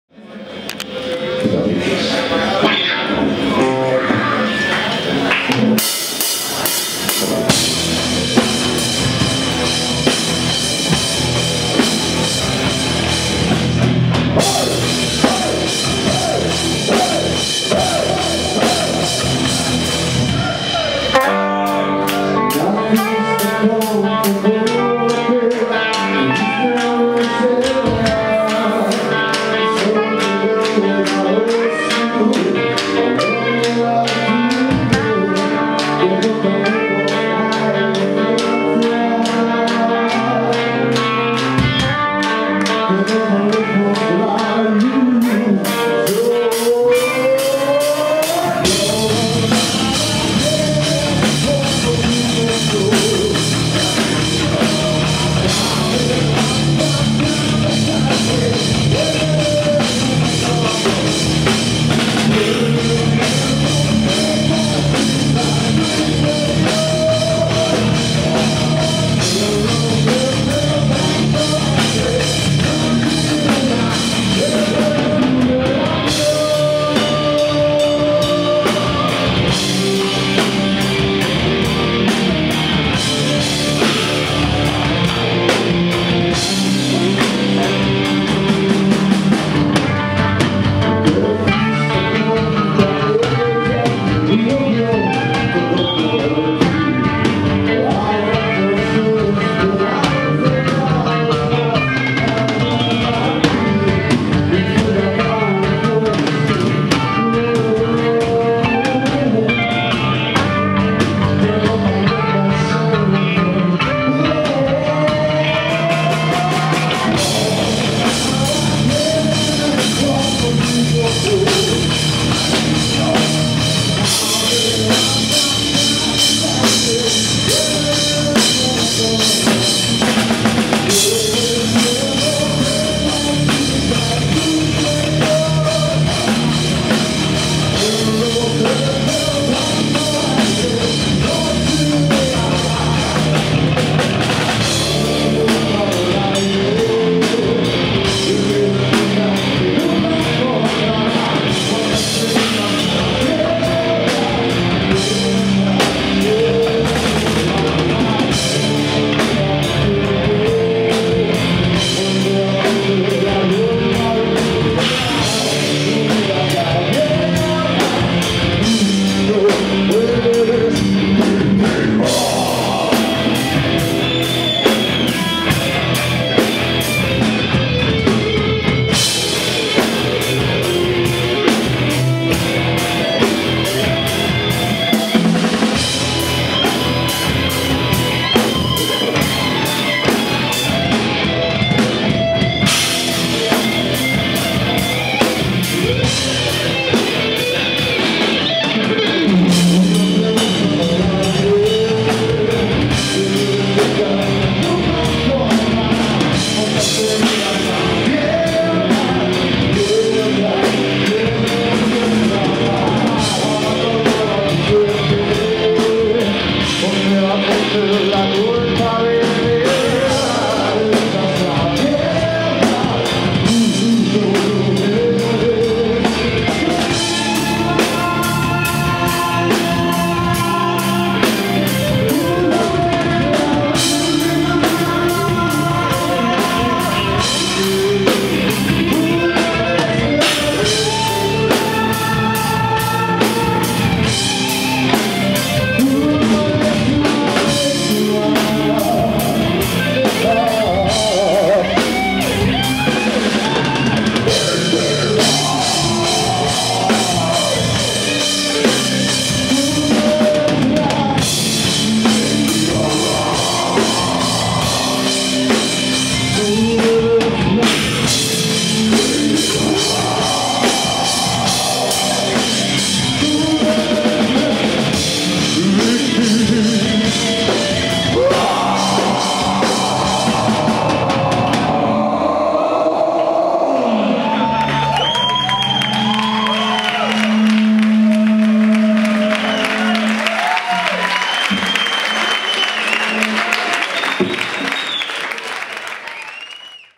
SHINOVA (live @ Honky Tonk) 4
Concierto de SHINOVA en la sala Honky Tonk de Tenerife el 7 de Mayo de 2010